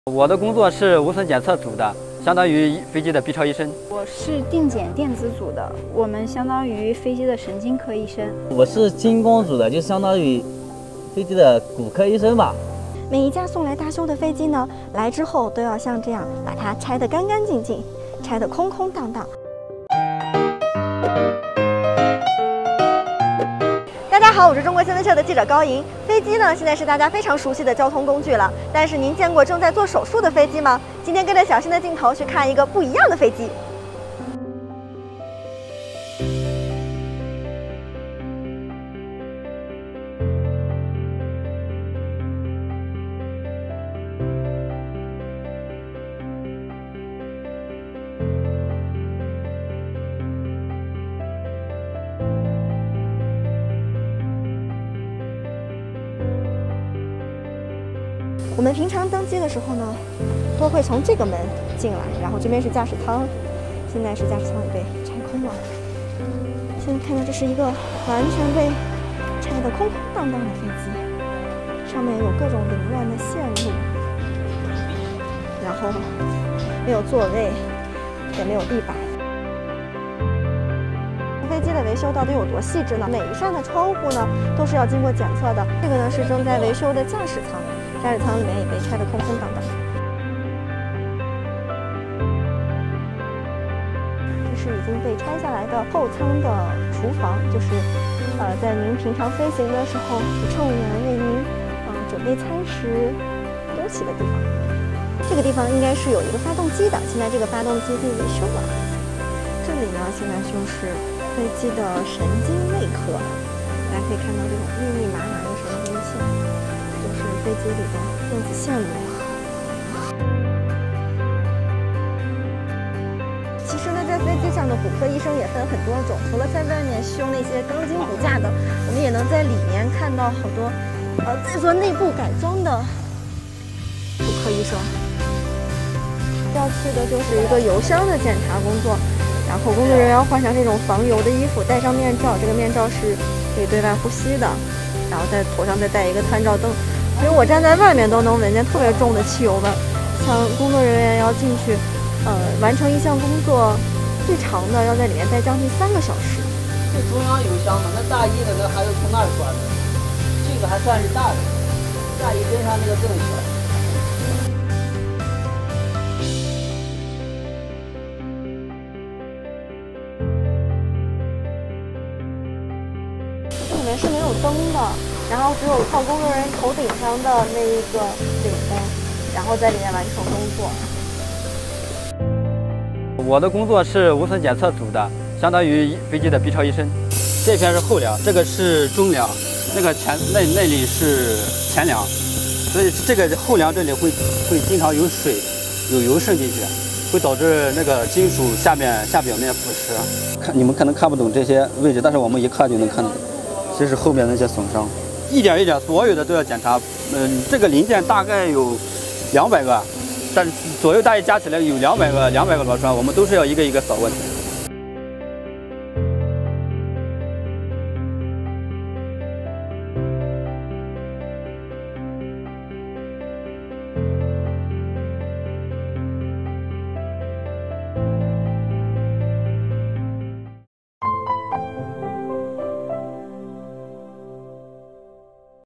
【小新的Vlog】飞机“素颜”什么样？小新揭秘飞机如何做手术
“我是无损检测组，被称为飞机的B超医生”，“我是金工组，相当于飞机的骨科医生”，“我是定检电子组，相当于它的神经科医生”……
        飞机是如今非常常见和便捷的交通工具，但是您见过被“掏空”的飞机吗？当原本干净整洁的客舱，被拆除了所有的座椅、内壳之后，“素颜”又是什么样子呢？
        在兰州中川机场的东航定检维修机库里，一架12年机龄的A320飞机正在进行最高级别的 “8C检”定检检修，这里又被称为飞机的“住院部”。 一次大型体检需要一百多位“医生”，分8至9个“科室”共同协作完成。不同组别之间既要“各司其职”，更需要“通力合作”。
        飞机医生们是如何给这个大家伙“做B超”、“接正骨”、“做美容”的呢？当机务从小三角口里钻进飞机油箱里，是什么感受？机库洗手间里的“洗眼器”又是做什么用的呢？跟着小新带你去揭秘